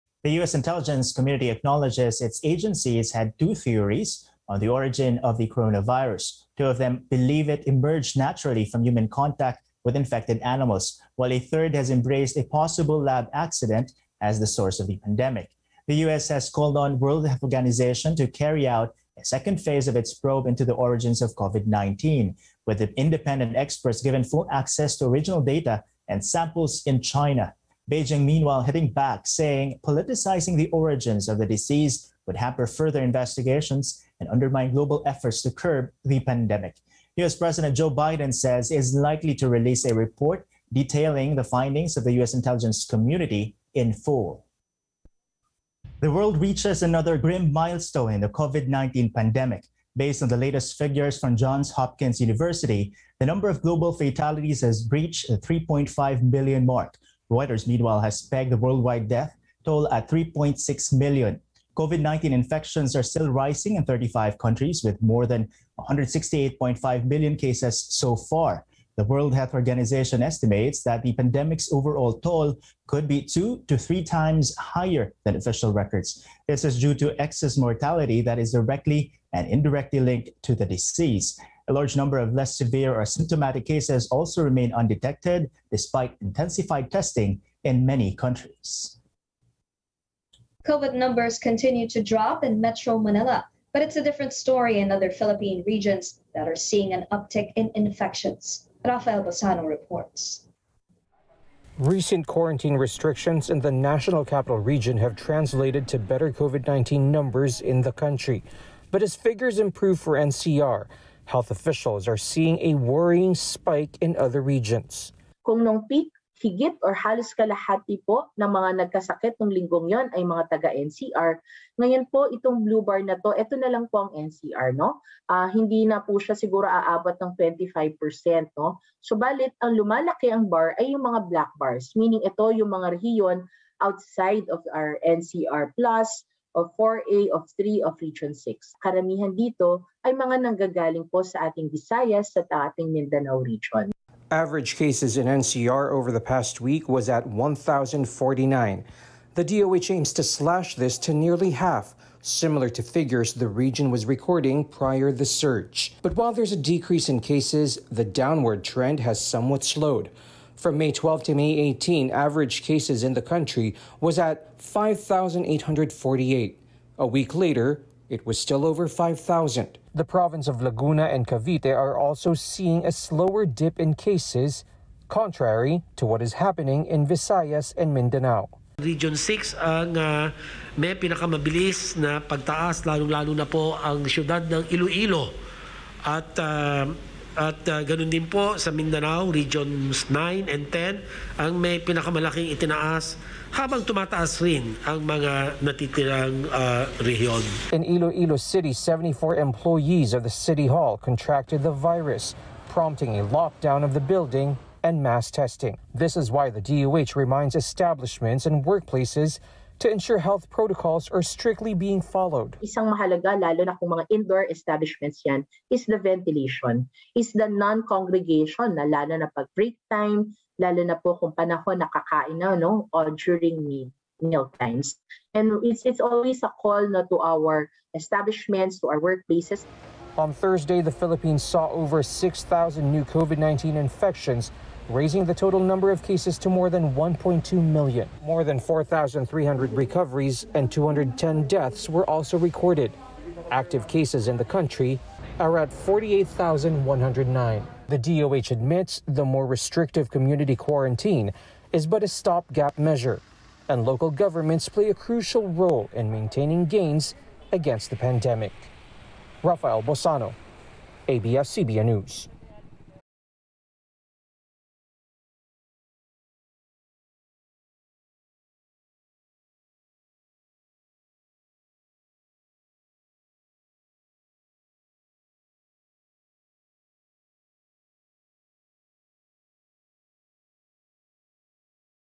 Natural or lab accident? U.S. Intelligence community acknowledges 2 theories of COVID-19 origin |ANC
Rundown: The U.S. intelligence community acknowledges its agencies had two theories on the origin of the coronavirus. Two of them believe it emerged naturally from human contact with animals, while a third has embraced a possible lab accident as the sources of the pandemic.